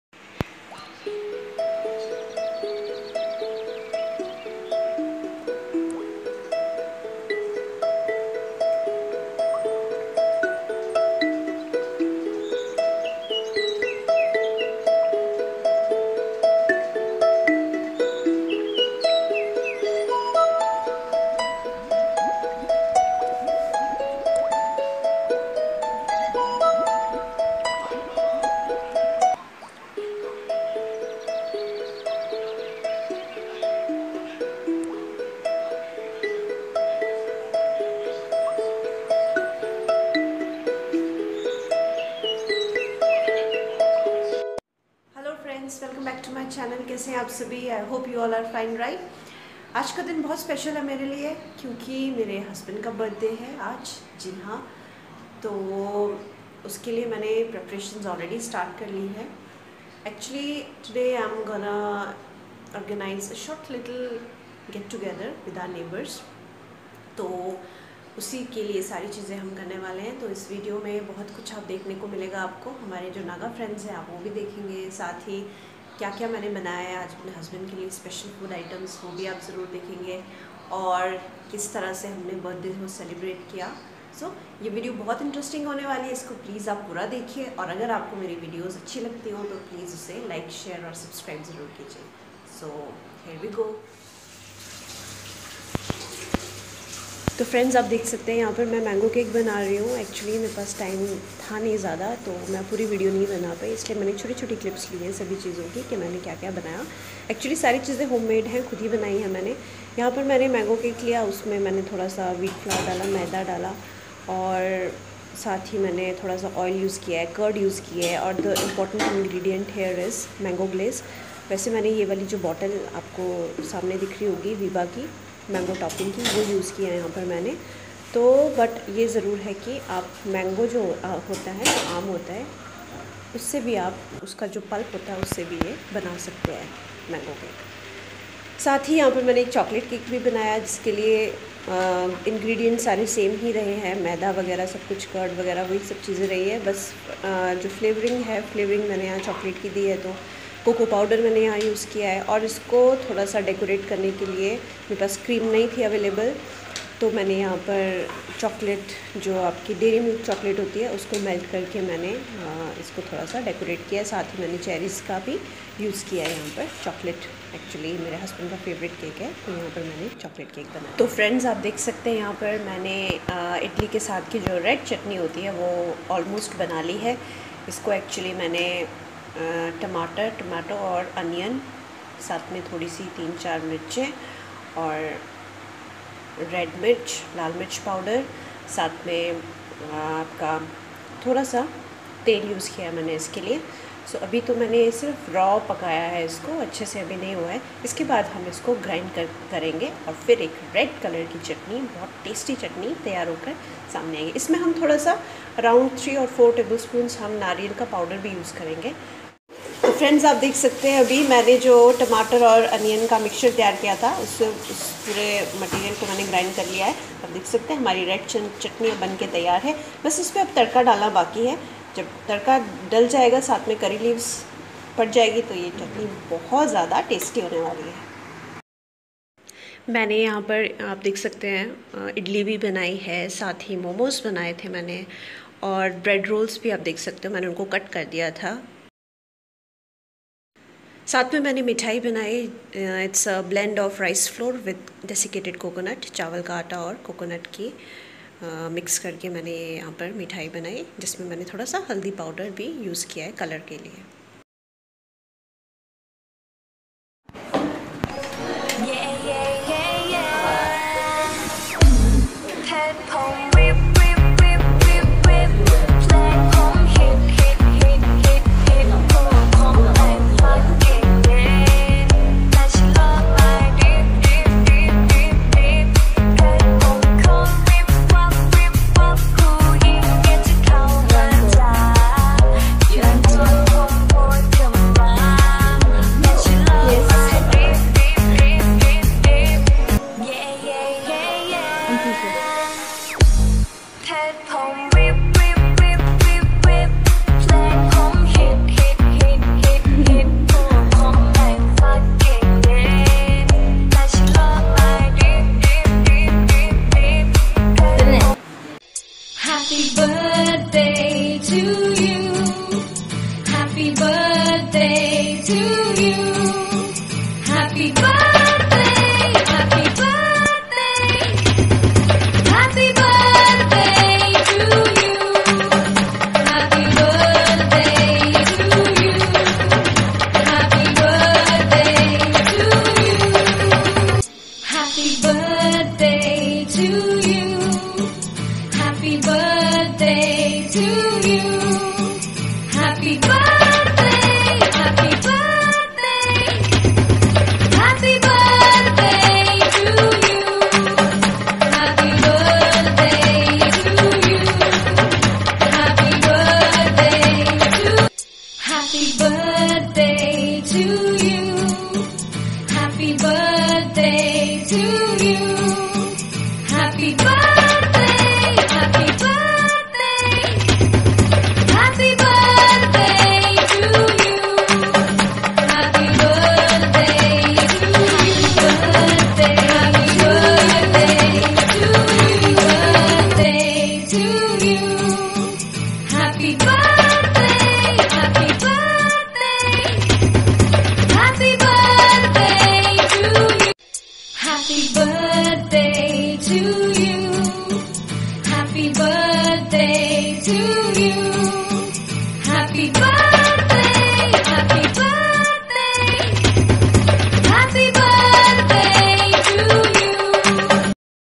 Birthday celebrations: with nagaland friends😄
secret recipes
cakes, mango and chocolate cake
idli with red chutney
momos with white sauce
peda as sweet dish
bread rolls